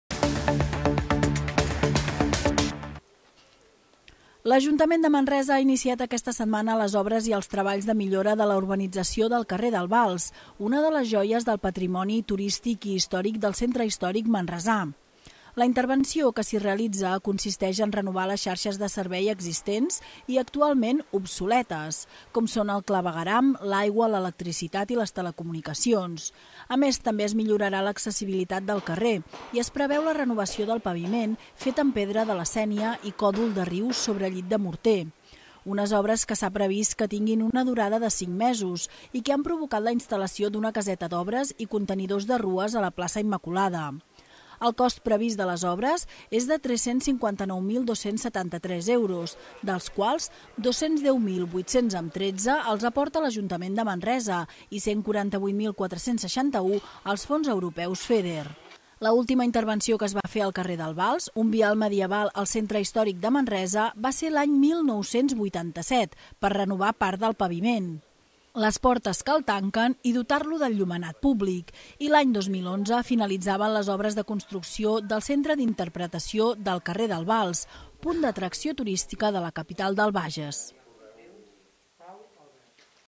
Obres al carrer del Balç a Manresa
L’Ajuntament de Manresa ha iniciat aquesta setmana les obres i els treballs de millora de la urbanització del carrer del Balç, una de les joies del patrimoni turisits i històric del centre històric manresà. La intervenció que s’hi realitza consisteix en renovar les xarxes de serveis existents i actualment obsolets, com són el clavegueram, l’aigua, electricitat i telecomunicacions. A més també es millorarà l’accessibilitat del carrer i es preveu la renovació del paviment fet amb pedra de la Sènia i còdol de riu sobre llit de morter. Unes obres que està previst que tinguin una durada de 5 mesos i que ha provocat la instal·lació d’una caseta d’obres i contenidors de rues a la plaça Immaculada.
El cost previst de les obres és de 359.273,62 euros, dels quals 210.813 els aporta l’Ajuntament de Manresa i 148.461 els Fons europeus FEDER.
L’última intervenció que es va fer al carrer del Balç, un vial medieval al centre històric de Manresa, va ser l’any 1987 per renovar part del paviment, les portes que el tanquen i dotar-lo d’ enllumenat públic i, l’any 2011 finalitzaven les obres de construcció de Centre d’Interpretació del Carrer del Balç, punt d’atracció turística de la capital del Bages.